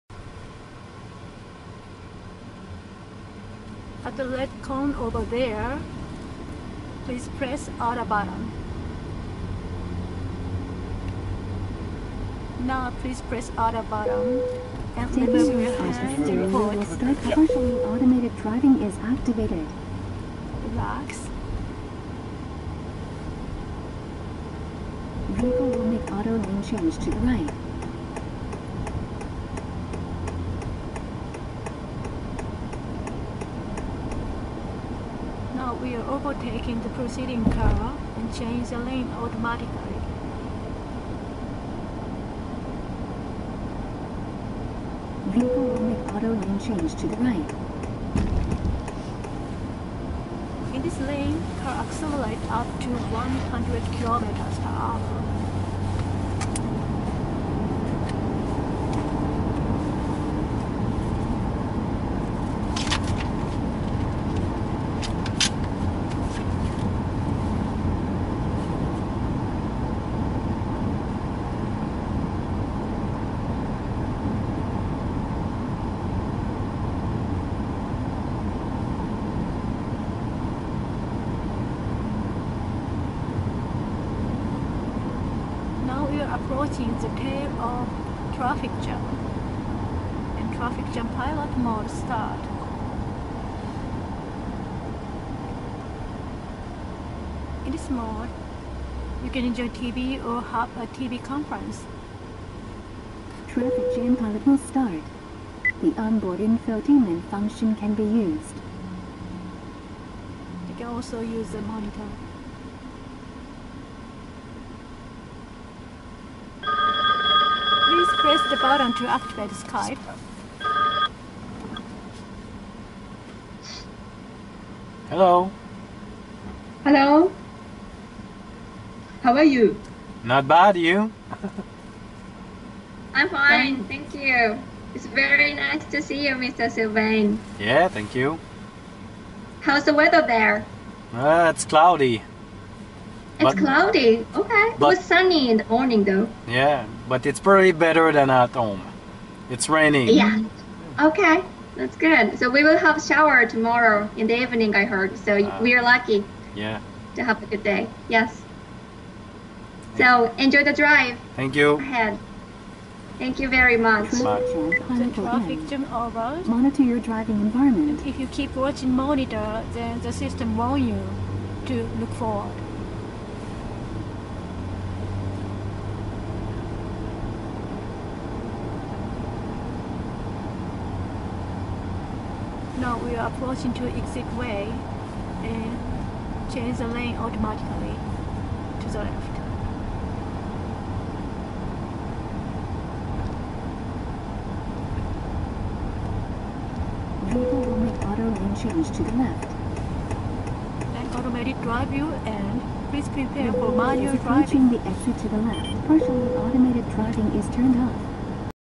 Honda - Conduite autonomne à l'essai
Honda - Test driving the Autonomous drive. Honda prévoit commercialiser une voiture autonome d'ici 2025.